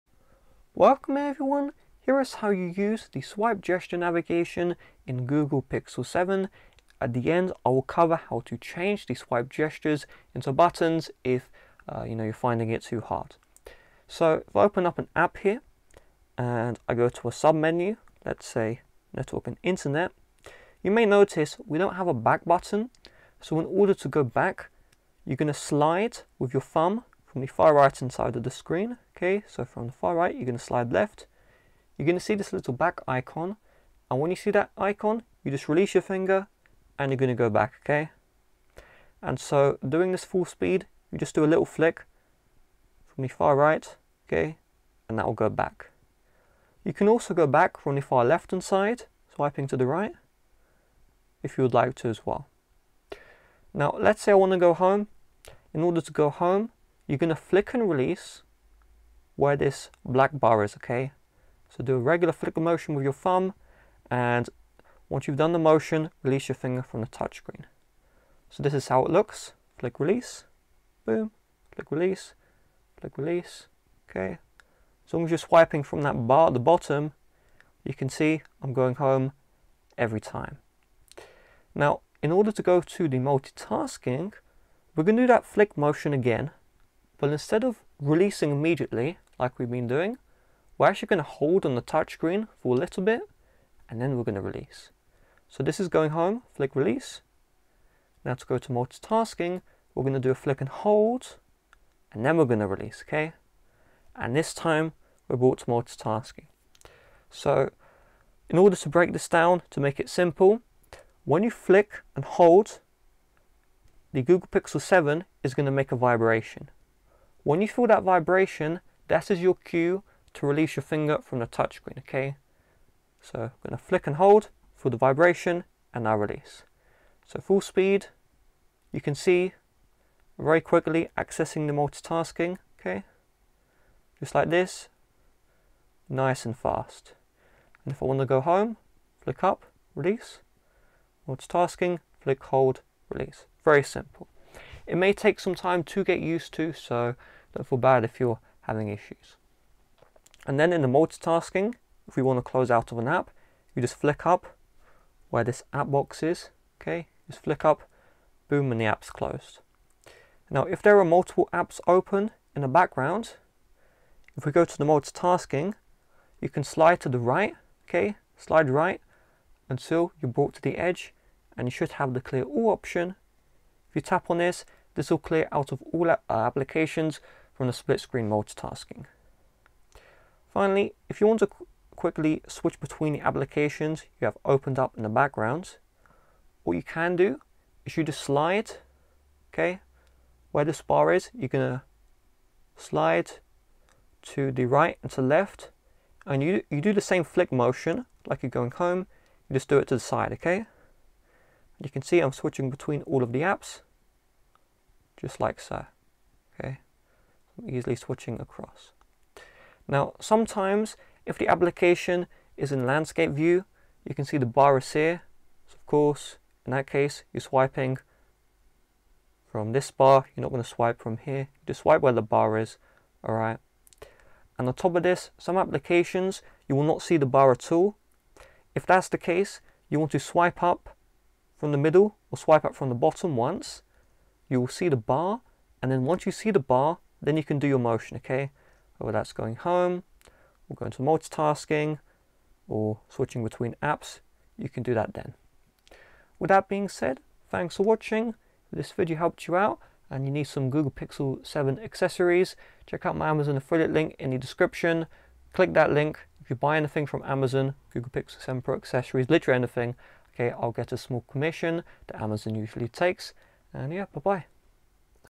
How To Use Gestures on Google Pixel 7 [Swipe Navigation Gestures]
Learn all of the swipe navigation gestures in Google Pixel 7. After watching this guide, each gesture used to navigate Google Pixel 7 should be easier to use.